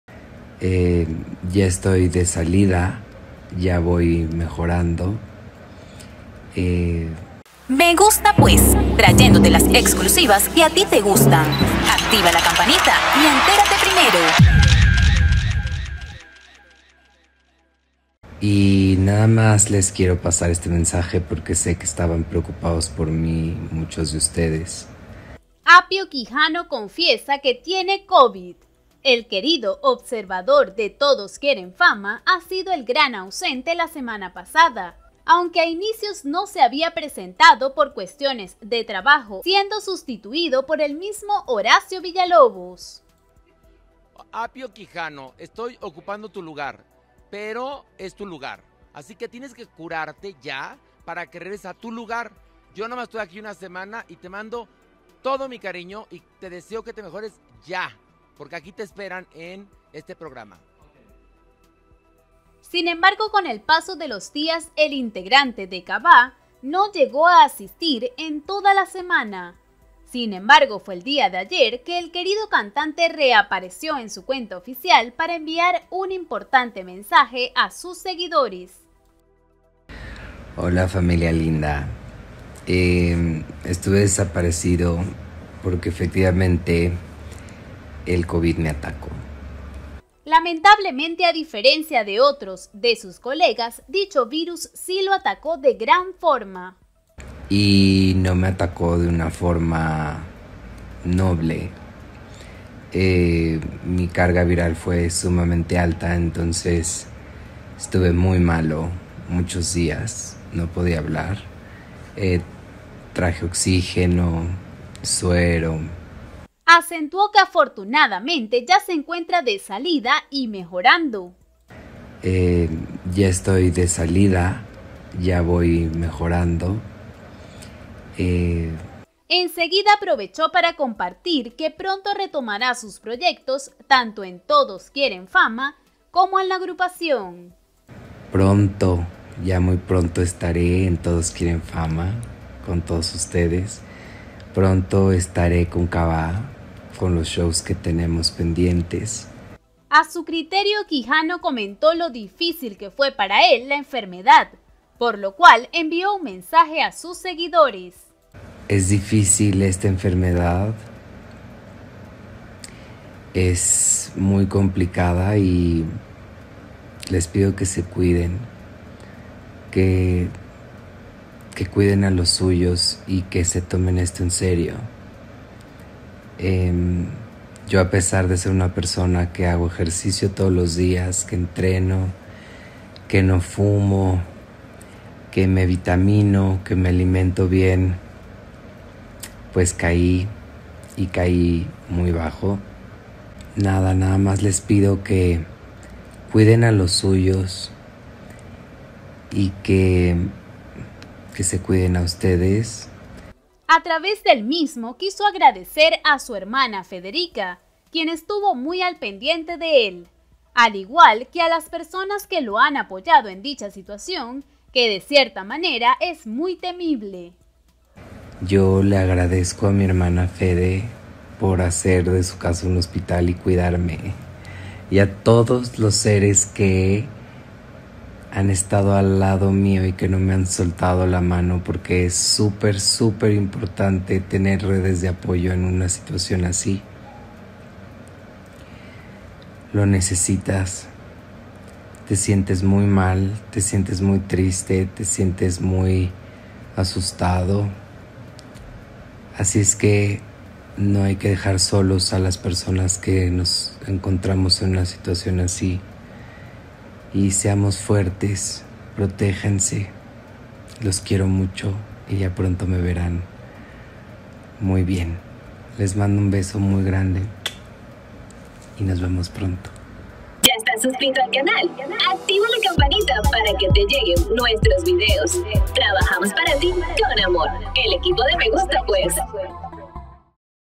Apio Quijano confiesa que tiene C0VID
El cantante se sumó a la lista de famosos contagiados.